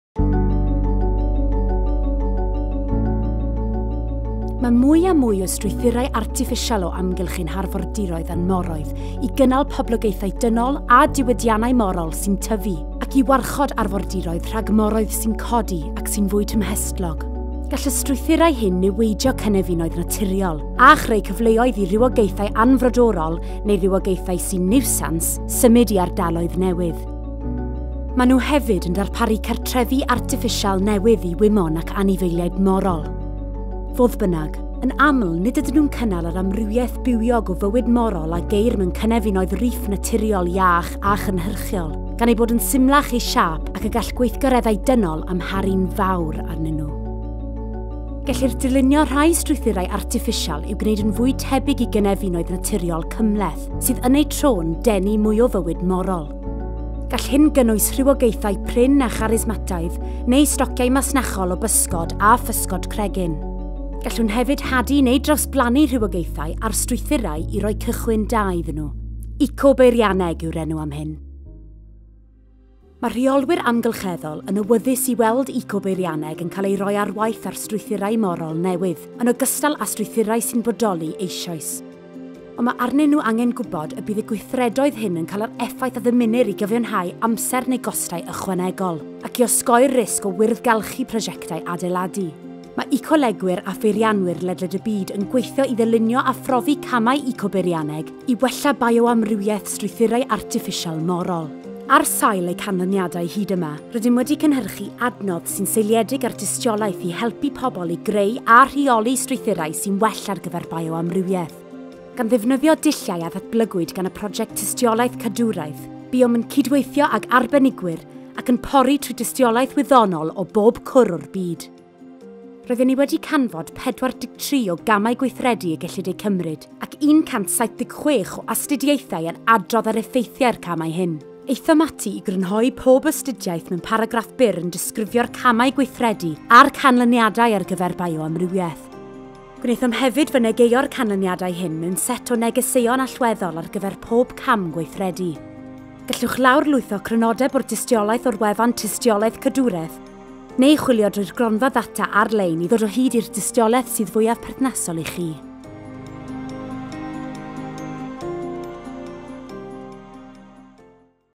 Eco-beirianneg: Gwella Bioamrywiaeth Strwythurau Artiffisial Morol
Mae gwella bioamrywiaeth strwythurau artiffisial morol o ddiddordeb cynyddol, a cheir pwyslais mewn polisïau ac awydd mewn diwydiannau i ymgorffori seilwaith cyfeillgar i fioamrywiaeth mewn datblygiadau morol ac arfordirol. Yn rhan o brosiect Ecostructure, a chan ddilyn dulliau a ddatblygwyd gan y prosiect Conservation Evidence, aethom ati i grynhoi’r dystiolaeth sydd wedi’i chofnodi ynghylch effeithiolrwydd y camau cadwraeth y gellir eu cymryd i wella bioamrywiaeth strwythurau artiffisial morol. Mae’r ‘synopsis o dystiolaeth’ yn crynhoi 43 o gamau y gellir eu cymryd, ac mae’n seiliedig ar 176 o astudiaethau o bob cwr o’r byd sydd wedi adrodd ynghylch effeithiau’r camau. Mae’r synopsis ar gael i bawb ei ddarllen ar wefan ‘Conservation Evidence’ a’i lawrlwytho fel dogfen PDF. Bydd yr adnodd yn galluogi ymarferwyr i wneud penderfyniadau ar sail tystiolaeth ynghylch yr opsiynau eco-beirianyddol gorau a’r opsiynau rheoli eraill gorau sydd ar gael ar gyfer eu hamcanion ar gyfer bioamrywiaeth seilwaith morol.

Gallwch ddysgu mwy am ymchwil Ecostructure ar neu gallwch ddod o hyd i ni ar twitter @ecostructure_.

Caiff ECOSTRUCTURE ei ariannu’n rhannol gan Gronfa Datblygu Rhanbarthol Ewrop drwy Raglen Gydweithredu Iwerddon Cymru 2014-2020.